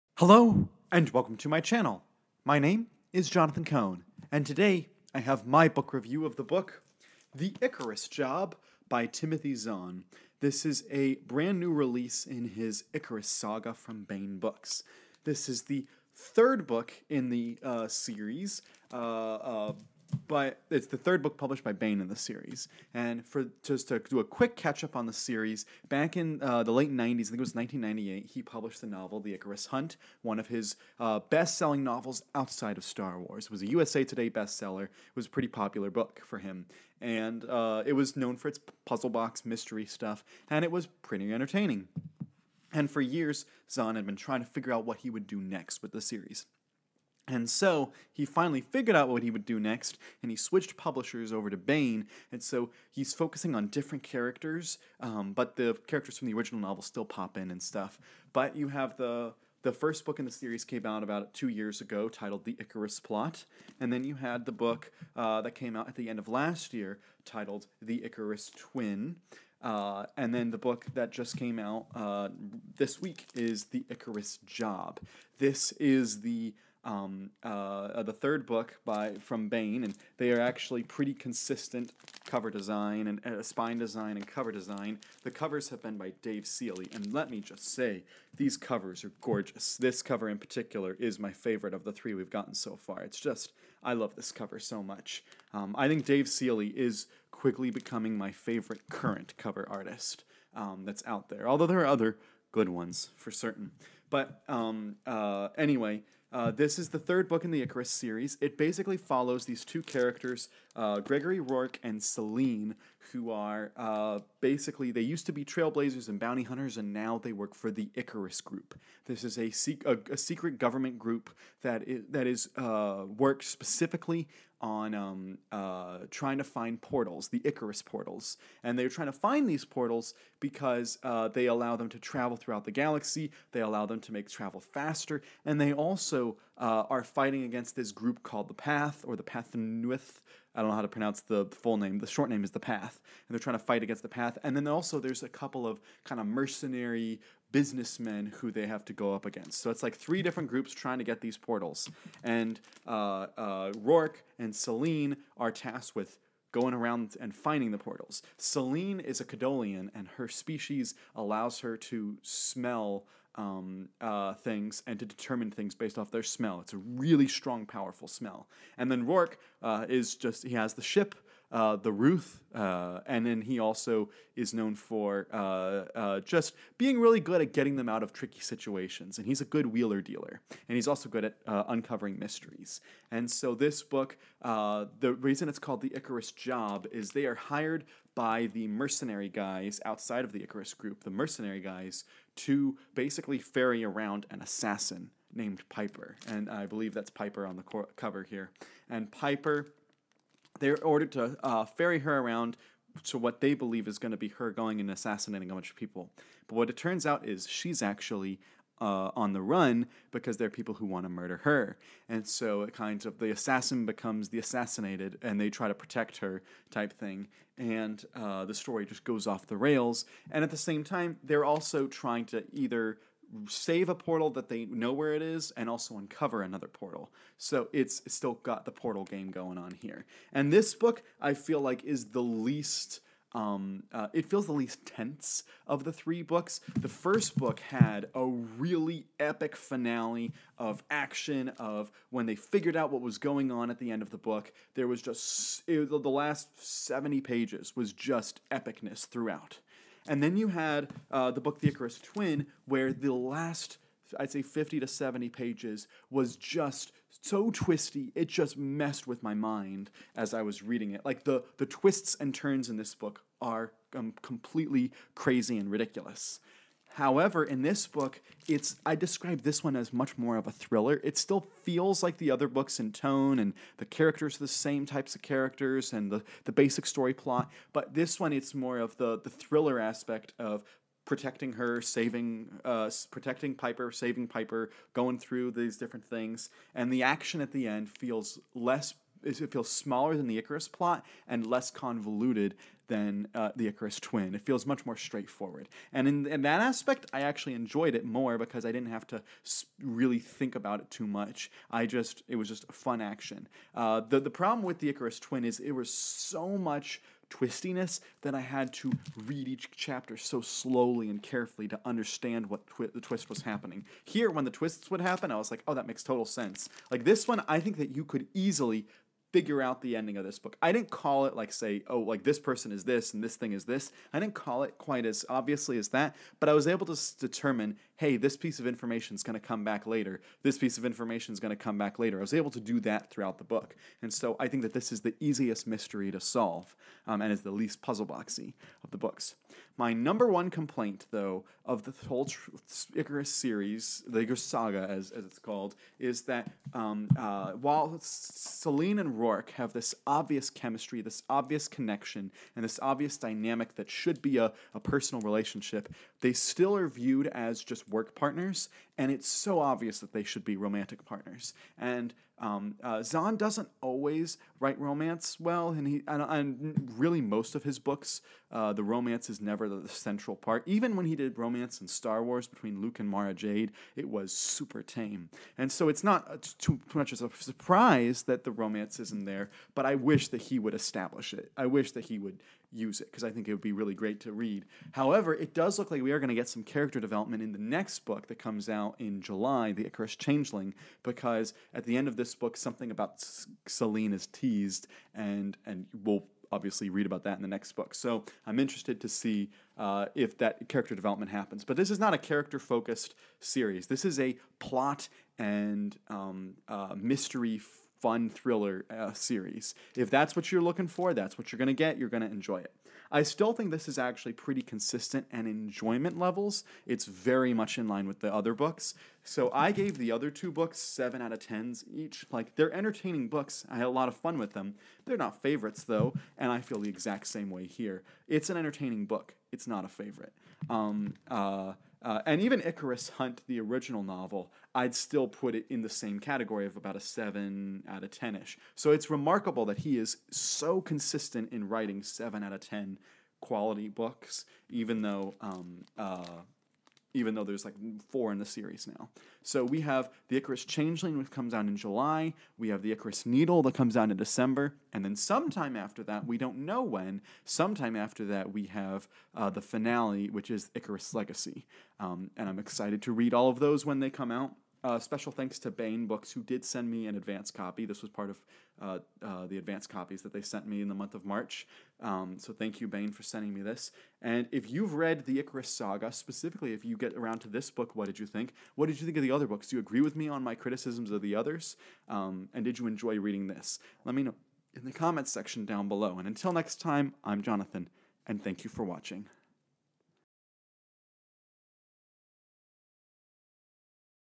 The Icarus Job Book Review (Timothy Zahn)
In this video, I discuss my review of "The Icarus Job" by Timothy Zahn, book three in his Mystery Puzzle Box "Icarus Saga". Enjoy